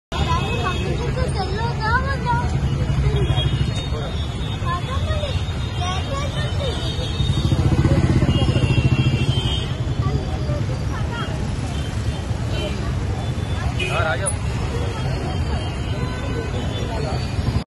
Minister Puvvada Ajay Kumar convoy meets with accident in Hyderabad | Chilly News
Minister Puvvada Ajay Kumar convoy meets with

accident in Hyderabad